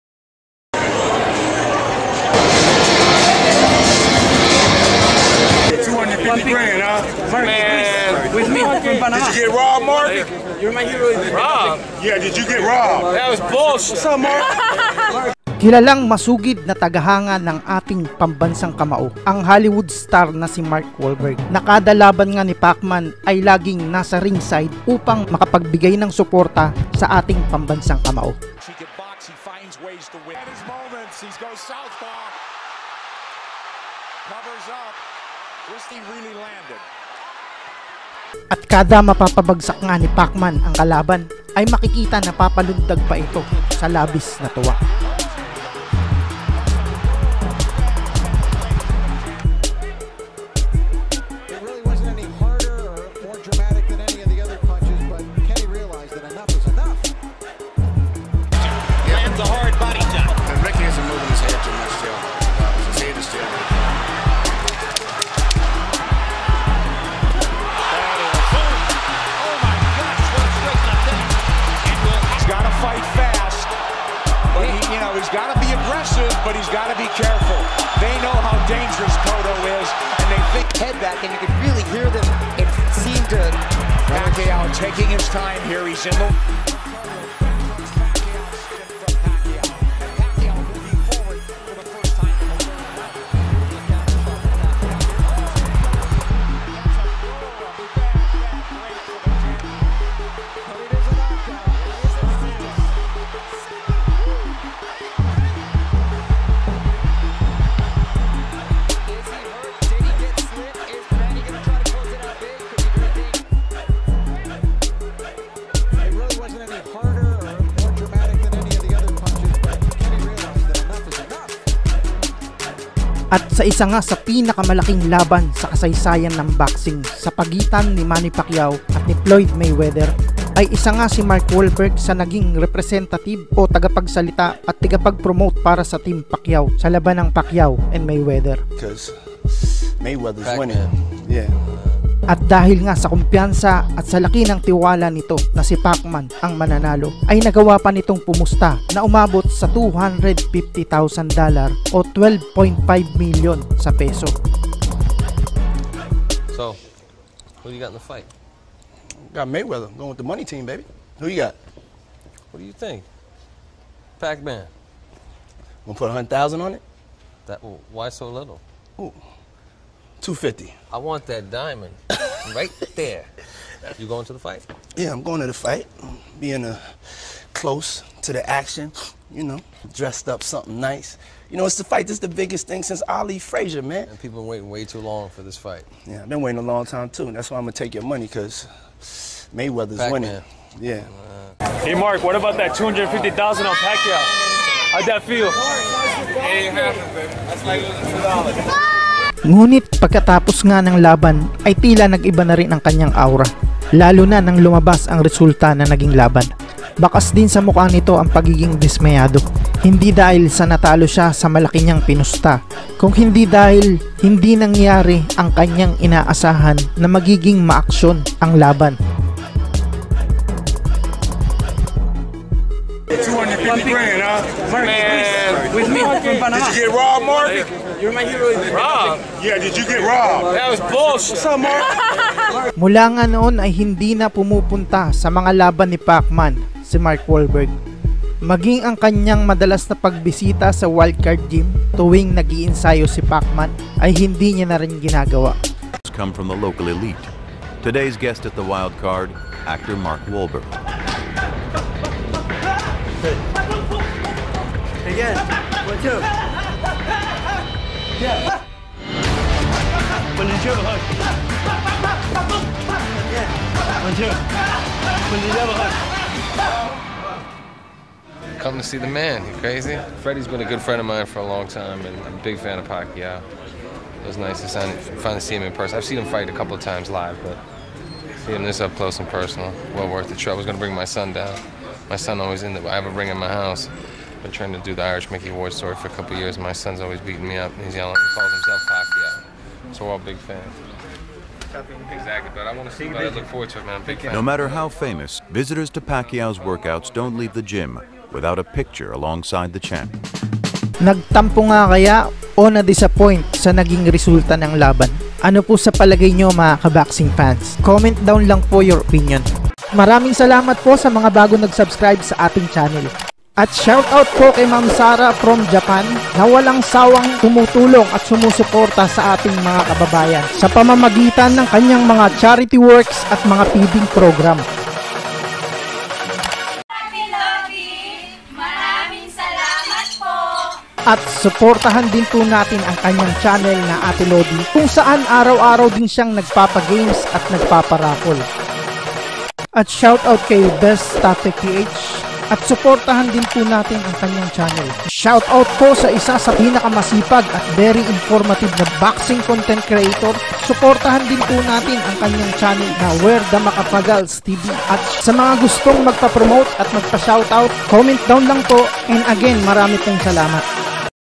Mark Wahlberg Nag Tampo Kay Pacquiao? | Matapos Matalo Ng $250K Sa Pacquiao vs Mayweather!
Mark Wahlberg May Tampo Nga Ba Kay Manny Pacquiao?

Matapos Matalo ng $250K Sa Labanang Pacquiao at Mayweather ay hindi na bumibisita sa mga laban ni pacquiao siMark Wahlberg  !

Every Time Manny Pacquiao have a fight Mark Wahlberg always visit him  in Wild Card Gym last footage (Dec 2, 2008)

Actor Mark Wahlberg watch pacman(MannyPacquiao) prepare for his fight for Delahoya this coming Sat. Dec 6,2008

Saturday, May 2, 2015 - Saturday's boxing 'Fight of Century' between Manny Pacquiao and Floyd Mayweather Jr. at the MGM Grand Garden Mark Wahlberg, who declares "that was bullsh*t" afterwards,

Footage Credit:

HBO Boxing
CBS Events